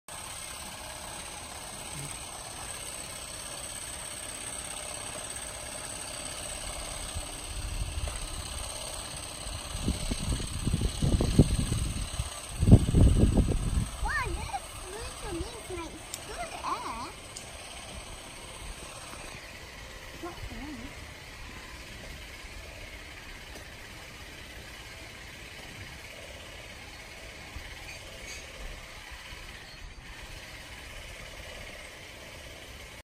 Extremely fast paced action keeping Retics
Ridiculous amount of fast paced herpetology action.